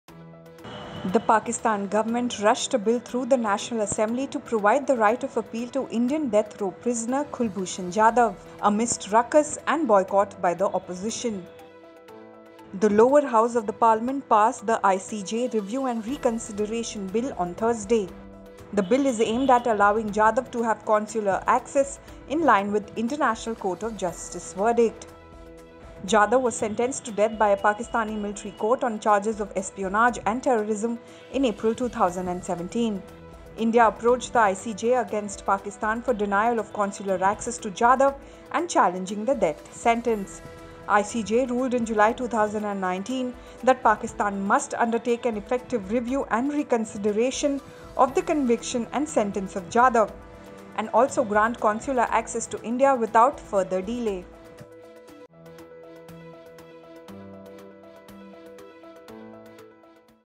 Pakistan passes bill to give right of appeal to Kulbhushan Jadhav
The Pakistan government rushed a bill through the National Assembly to provide the right of appeal to Indian death-row prisoner Kulbhushan Jadhav amidst ruckus and boycott by the Opposition.

To stay updated, download the editorji App:
Editorji.com/download